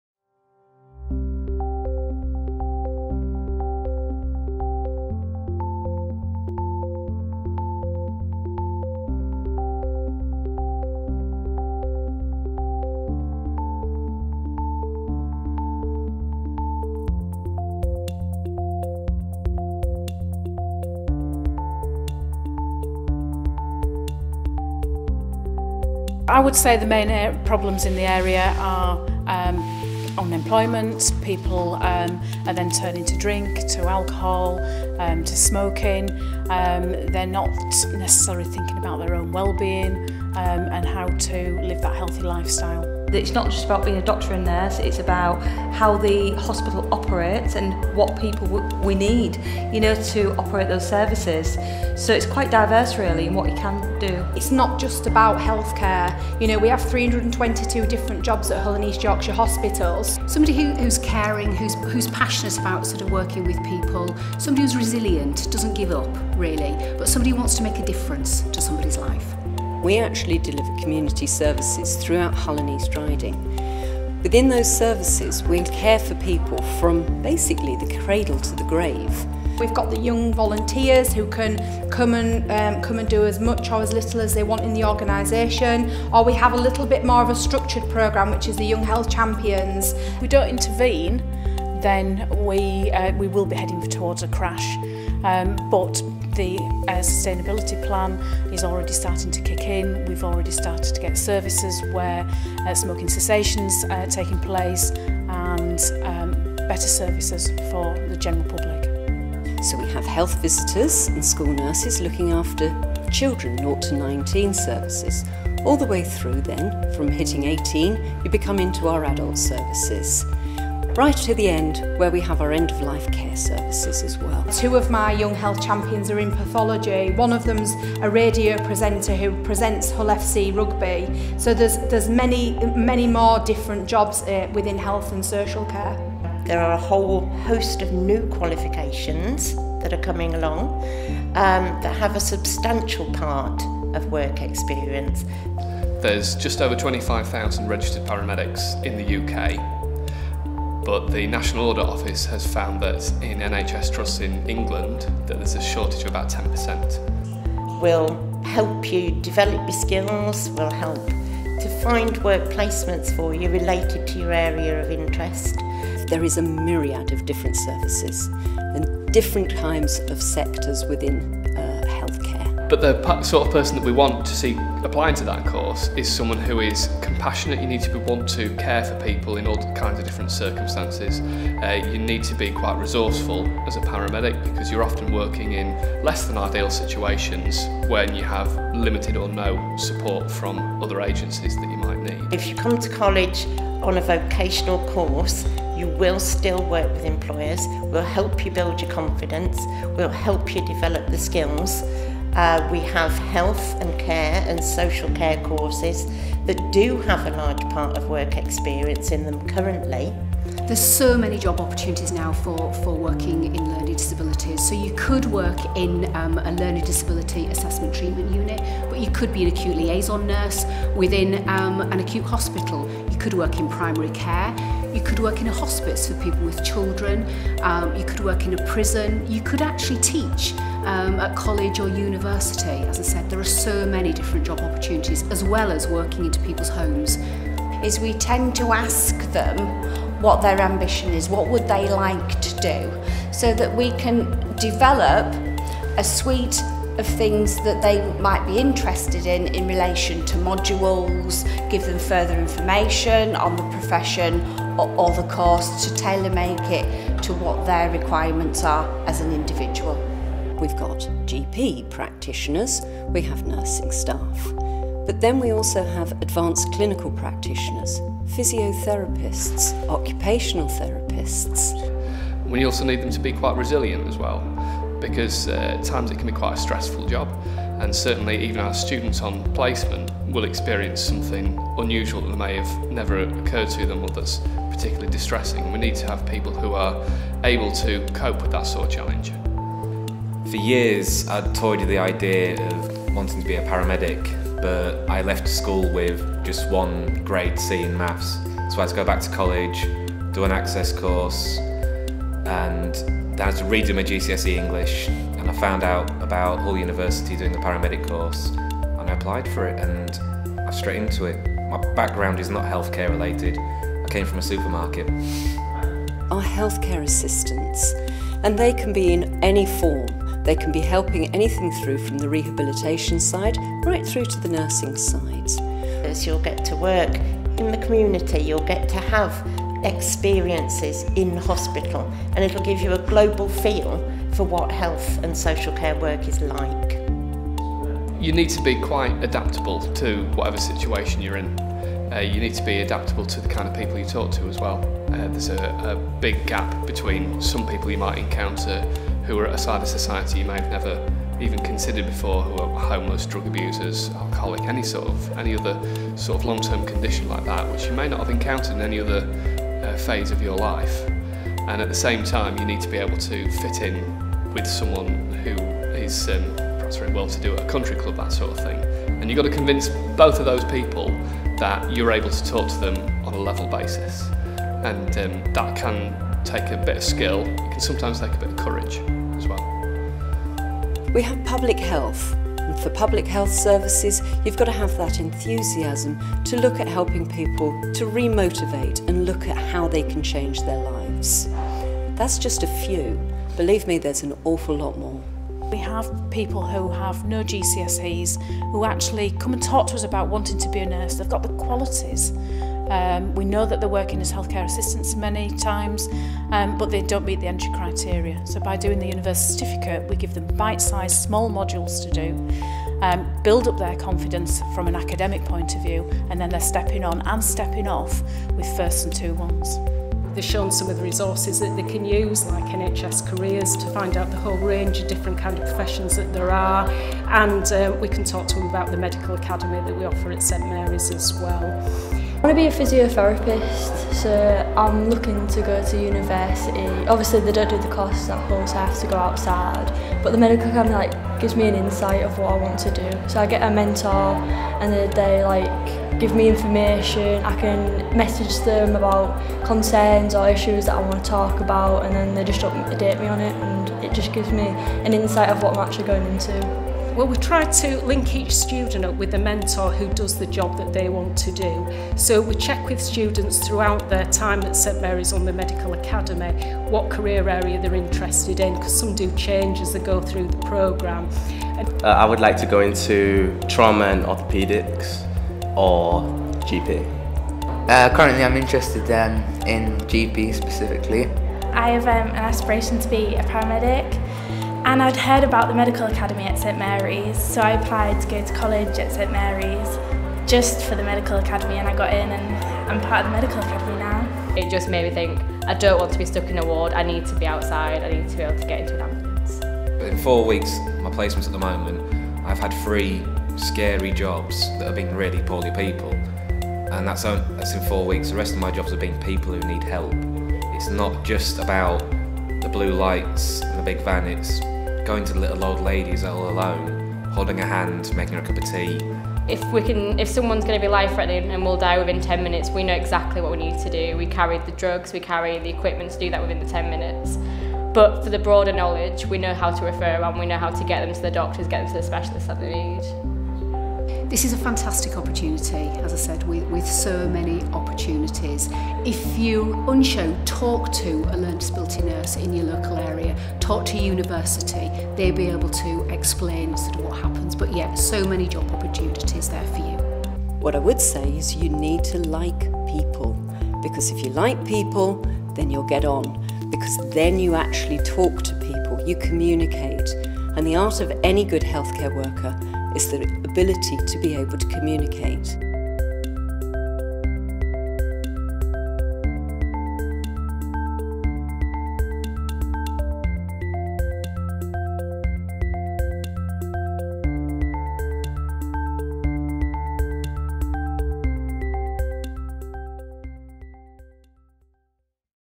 Get a career in the Health Sector with LMI Humber 2018
Get an insight into the employment opportunities, routes into employment and skills & qualifications needed in key sectors in the Humber from LMI Humber